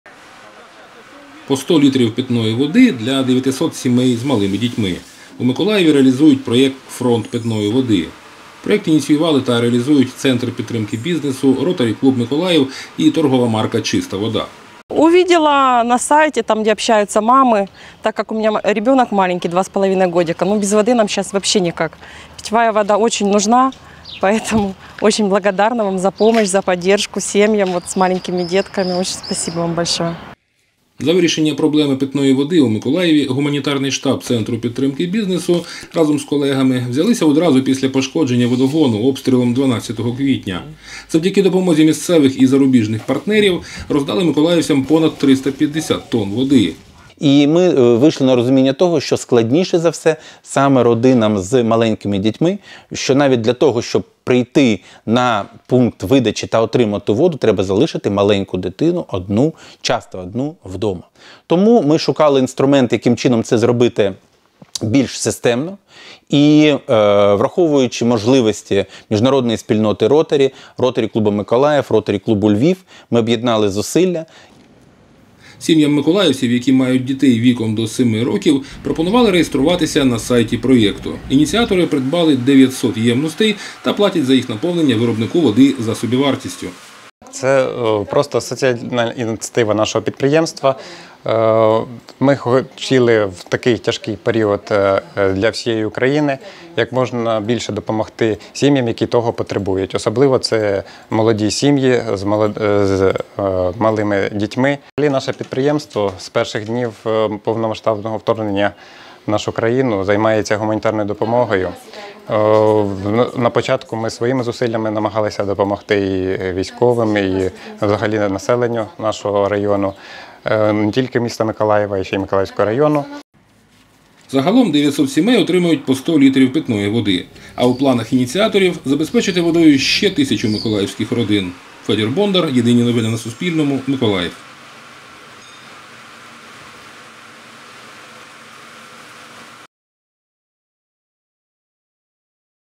У Миколаєві реалізують проєкт «Фронт питної води»
По 100 літрів питної води для 900 сімей з малими дітьми – в Миколаєві реалізують проєкт «Фронт питної води».
Автор-оператор – Федір Бондар.